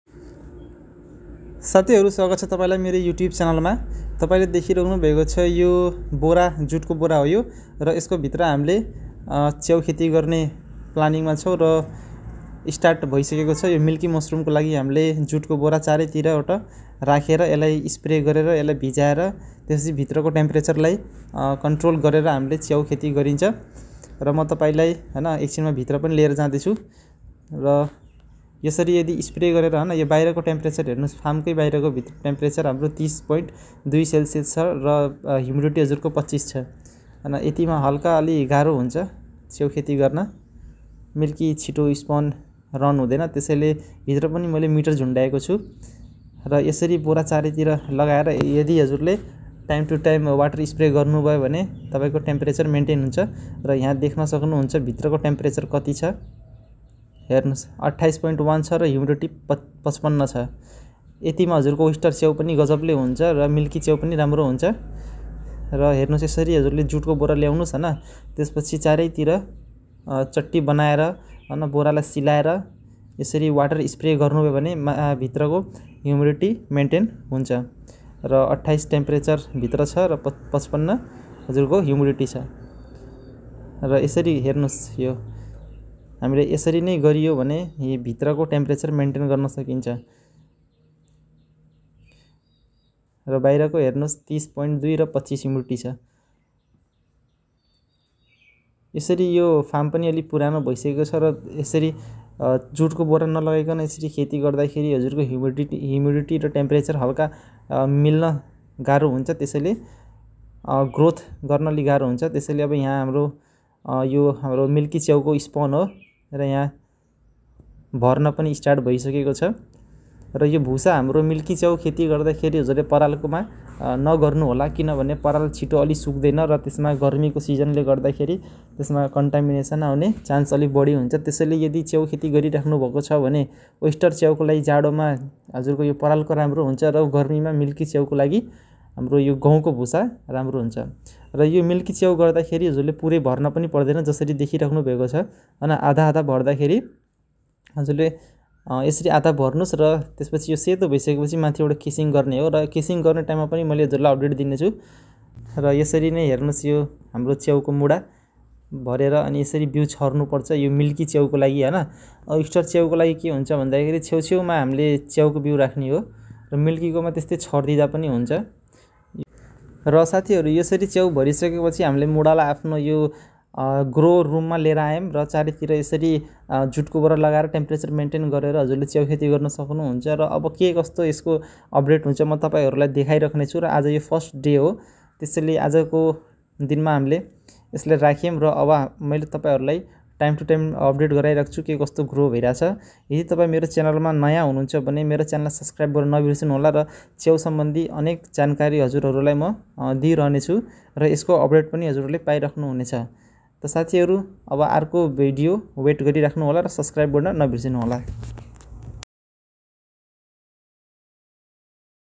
Milky Mushroom Cultivation: Mastering Temperature Control for Successful Farming
Learn how to successfully cultivate Milky Mushrooms by controlling the temperature at your farm! This video covers everything from the basics of Milky Mushroom farming to tips on maintaining the ideal environment for optimal growth. Enhance your farming techniques and increase your mushroom yields with these expert strategies.